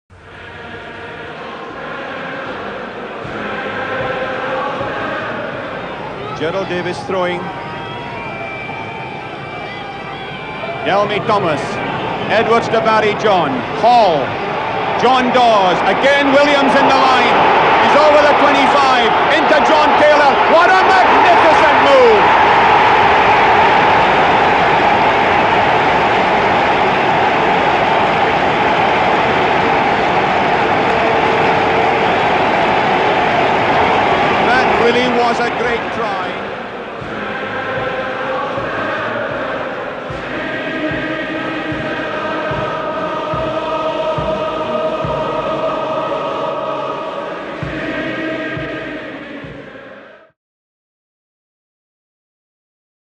John Taylor - 1971 Five Nations vs Scotland at Murrayfield
1971 February 6 - Wales vs Scotland at Murrayfield

Wales 19 - Scotland 18

John Taylor scores one of the all time great tries by a flank forward

WELSH TEAM
15  FB    JPR Williams
14  W     Gerald Davies
13  C      John Dawes (Captain)
12  C      Ian Hall
11  W     John Bevan
10  FH    Barry John
9    SH    Gareth Edwards
1    P       Denzel Williams
2    H       Jeff Young
3    P       Barry Llewelyn
4    L       Mike Roberts
5    L       Delme Thomas
6    F       Dai Morris
7    F       John Taylor
8    N8    Mervyn Davies
Replacements
17    Phil Bennett
18    Ray Hopkins
19    John Lloyd
20    Norman Rees
21    Derek Quinnell